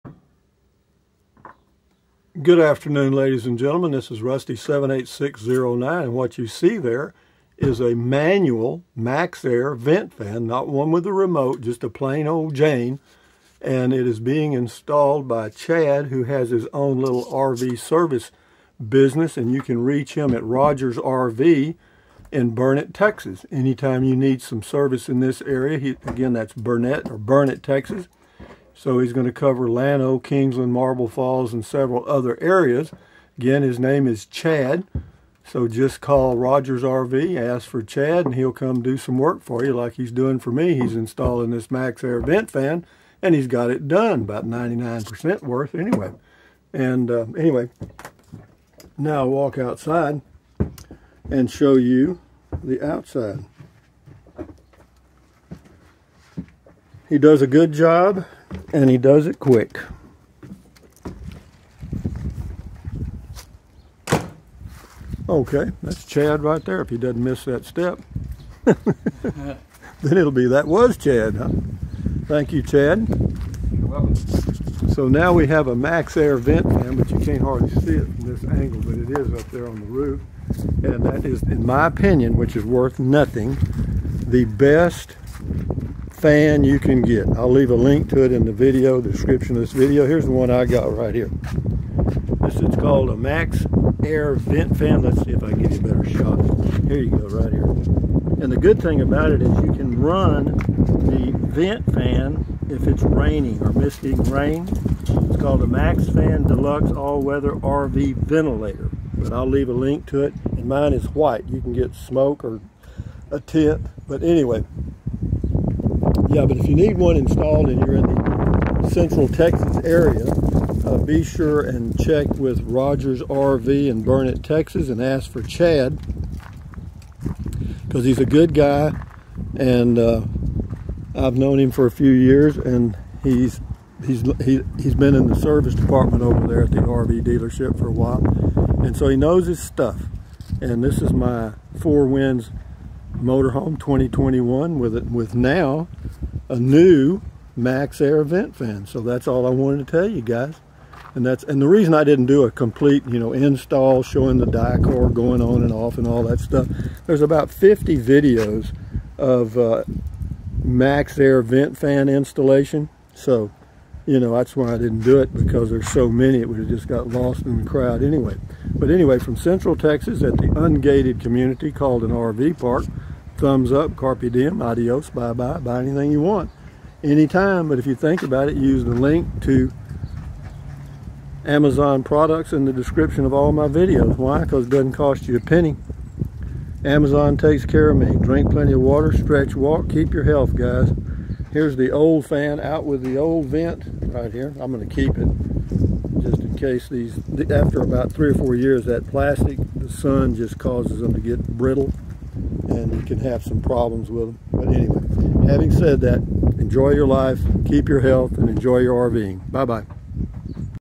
MY CLASS C MOTORHOME...MAXXAIR VENT FAN INSTALLED!!!
This is not a detailed vent install video. Just me "polishing the diamond"...as in getting my new home set up my way! thank you!!!